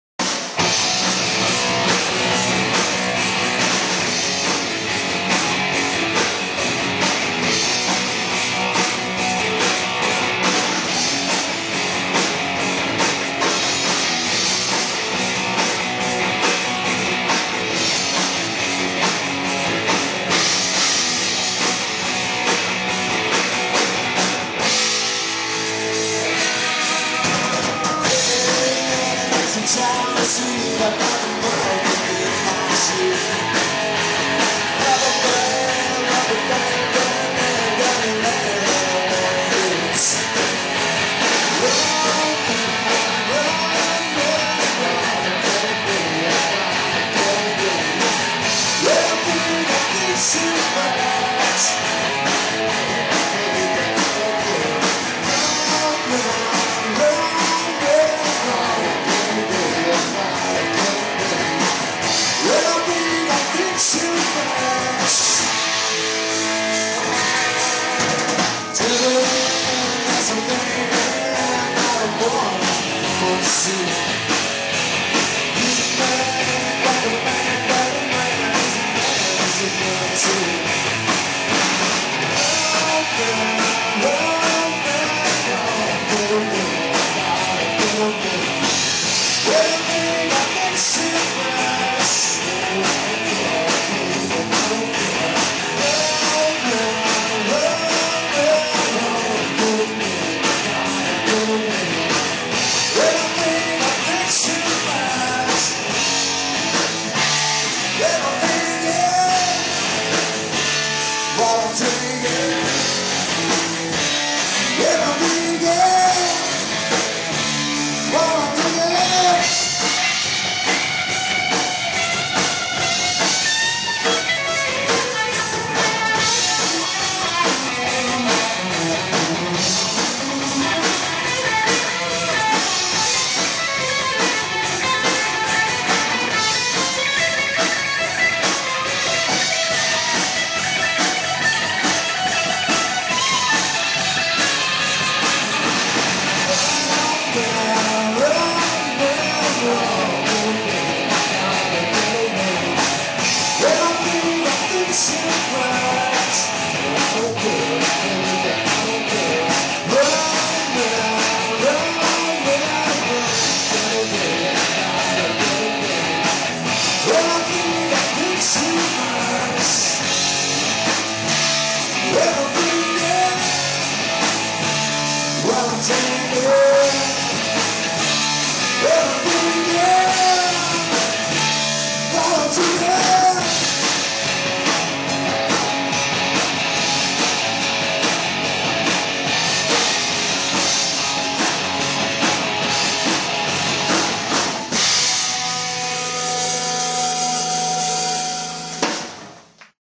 Project X at GoOdFeLLaS
Project X featuring Neriah Hart on guitar and Pete"the Beat"Burger on drums